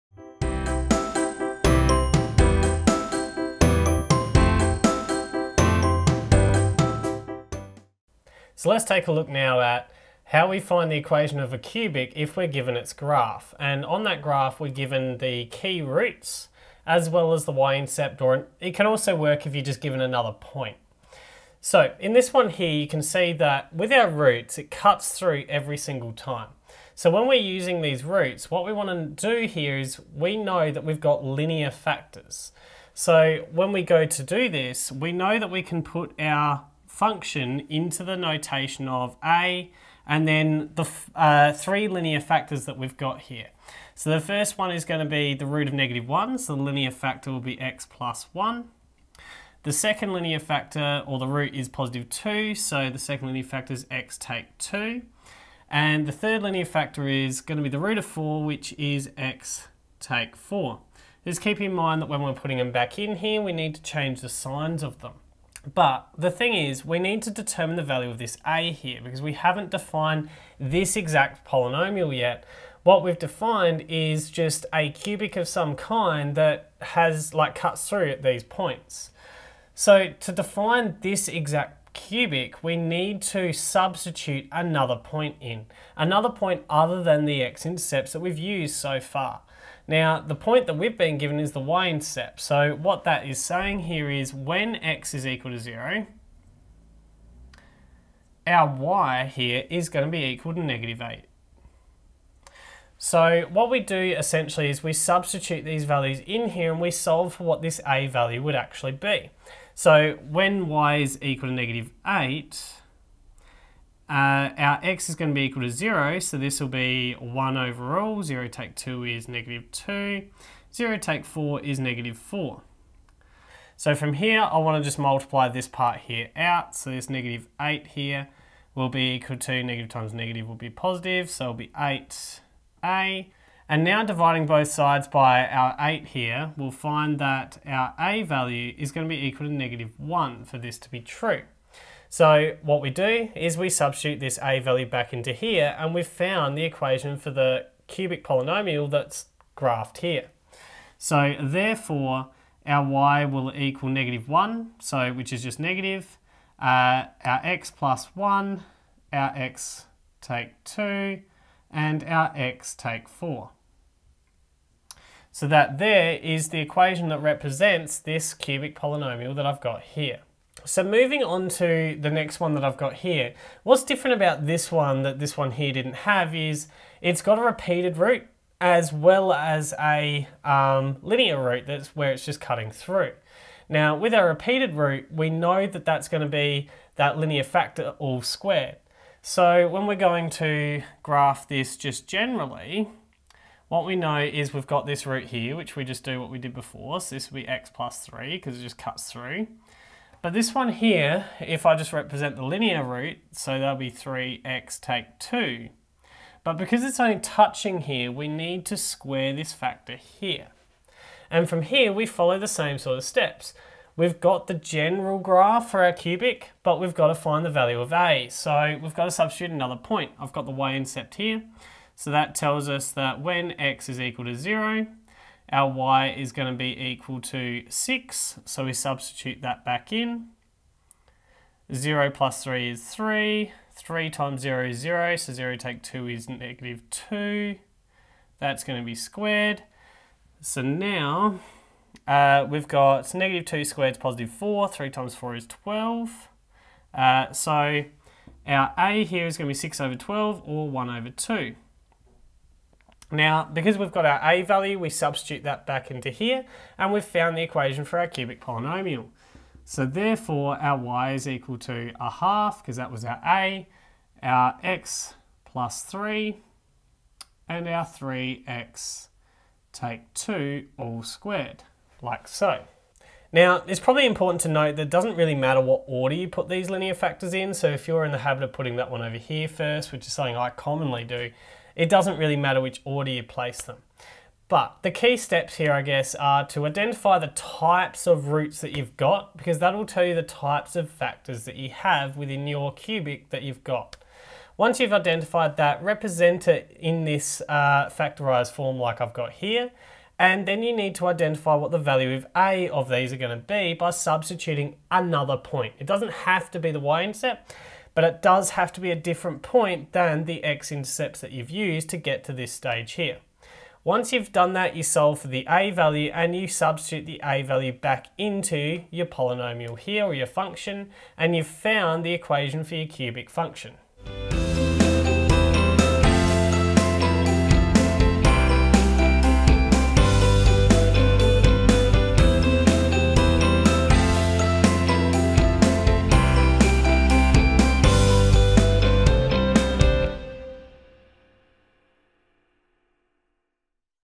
Finding Cubic Functions From Its Graph
In this video I demonstrate how to find the function of a cubic polynomial from its graph.